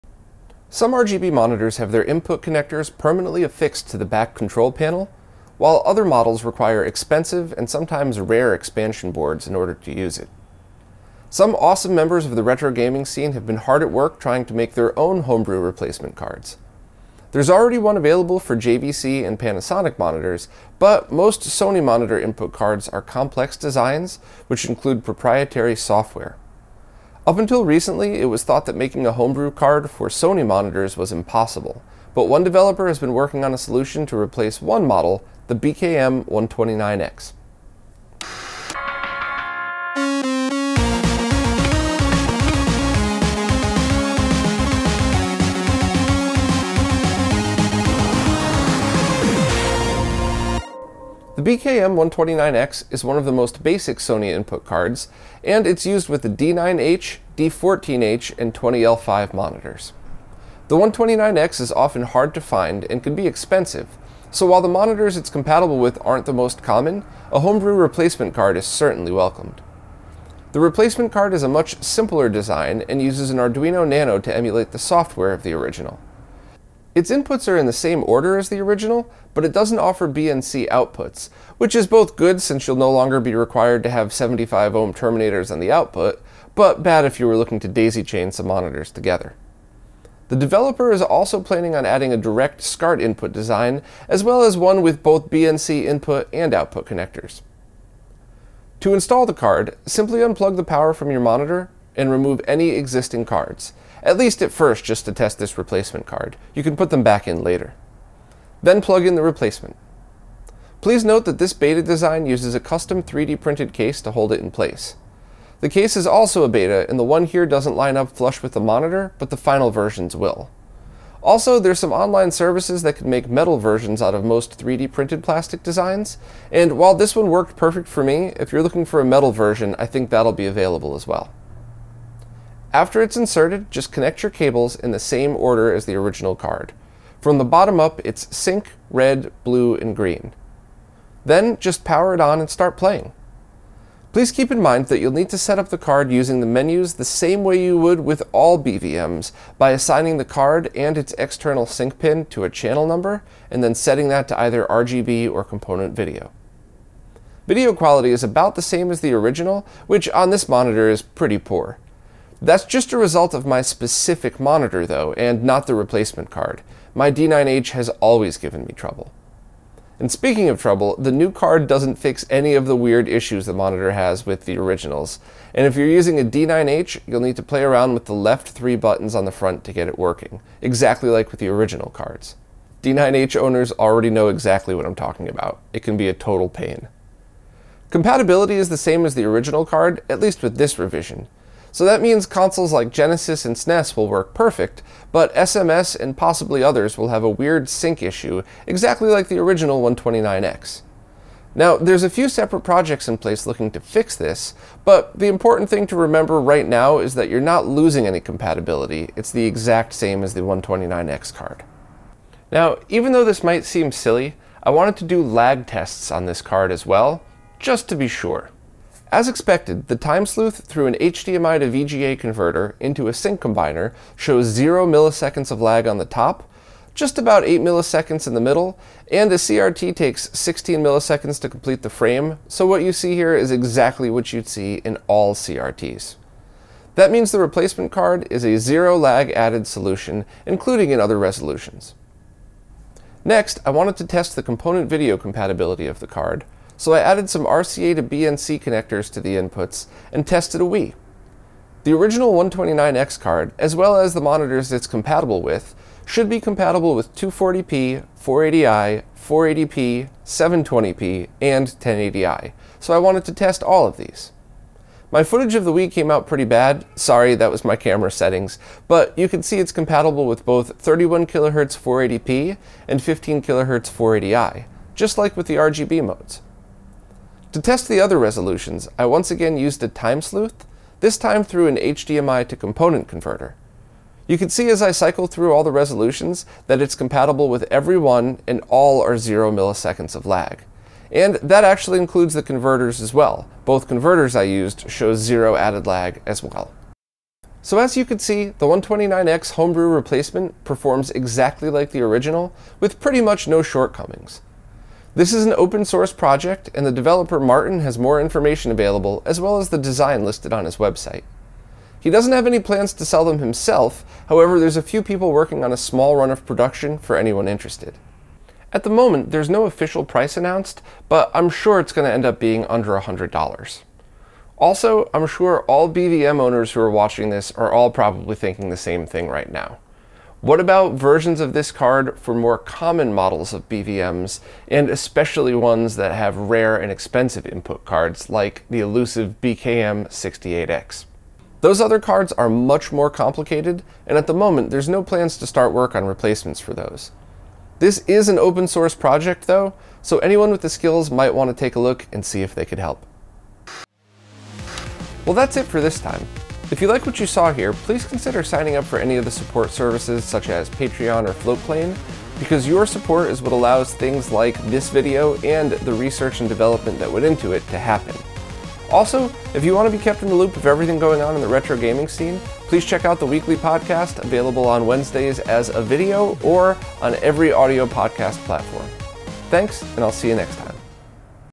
BKM 129x Homebrew Card
I was able to test an open-source project that provides a homebrew replacement for the Sony BKM-129x RGB/YPbPr input card used in select Sony BVM monitors:  The BVM D9H & D14H and the PVM 9L2. & 20L5 (secondary slot).  This card was made by reverse-engineering the original and using an Arduino Nano to emulate the software.  I tested both RGB and Component Video signals across all supported resolutions (240p-1080i) and all worked perfectly without adding any interference or lag.

Hopefully other people will become interested in this project and help migrate it to different BVM's that could absolutely use the support.  For now, the developer doesn't plan to sell it himself, but one person is currently make a small, group production run for anyone interested.

And here's the previous project mentioned at the beginning of the video:
JVC Monitor SCART Input Card:
JVC Montior BNC Input Card:

If you liked this video, please consider supporting in any way

All equipment used to shoot this video can be found